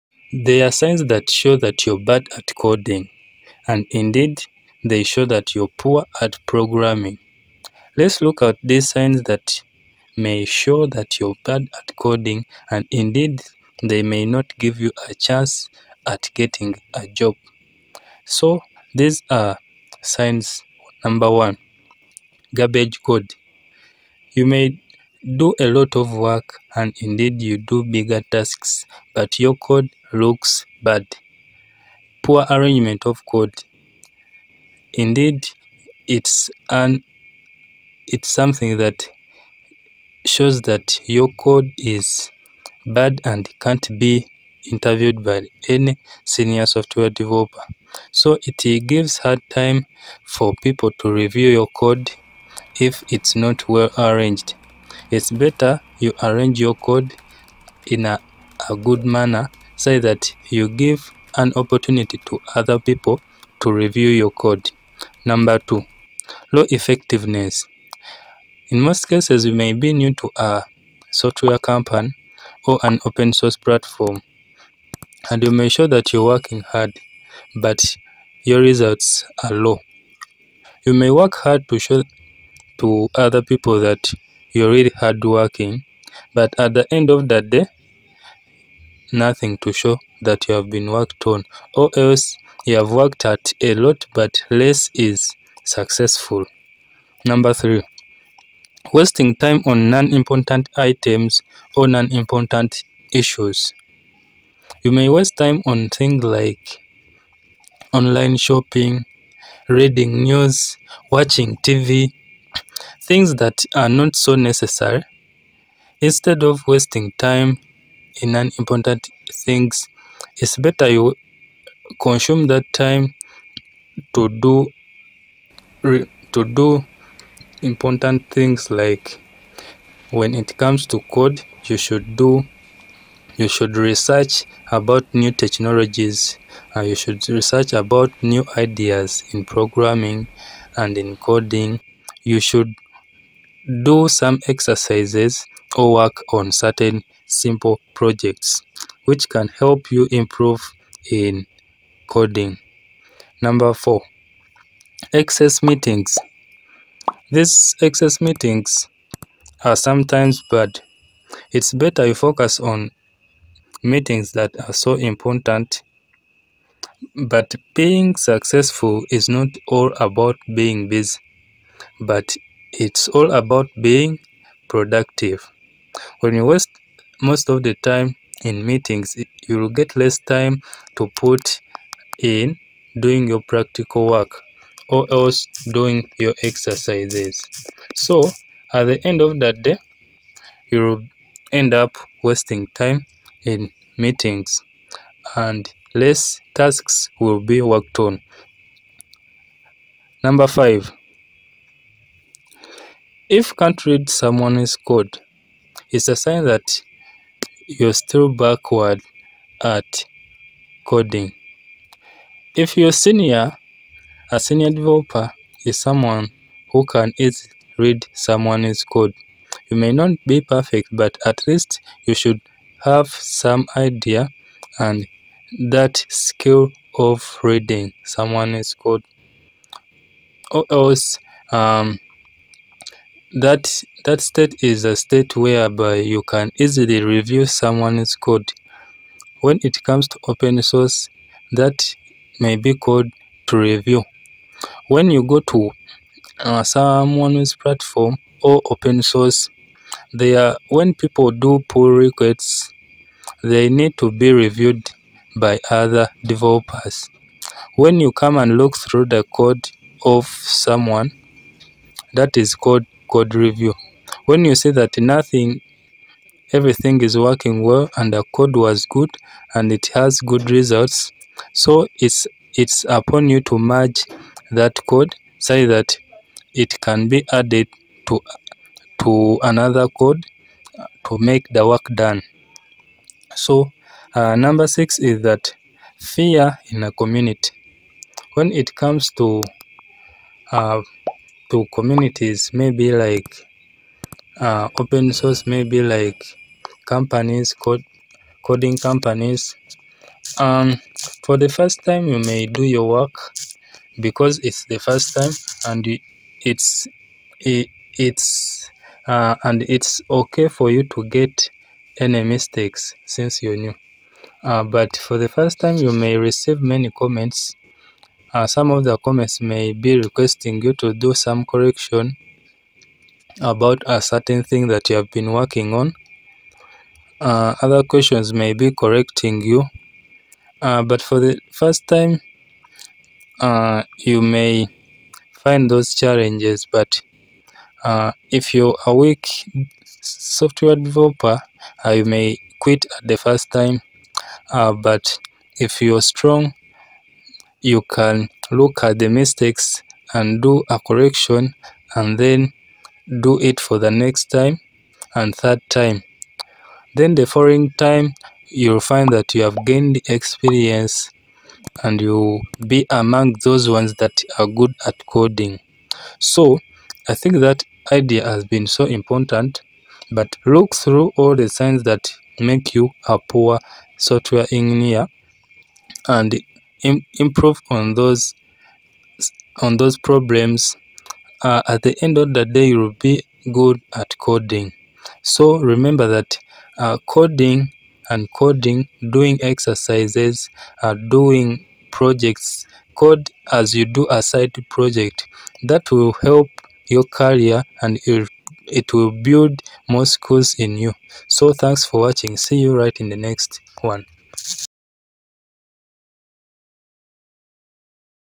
Signs of Poor Programmers ( Bad Software Engineer )
Signs of a Bad Programmer,  These are signs of unexperienced software engineers.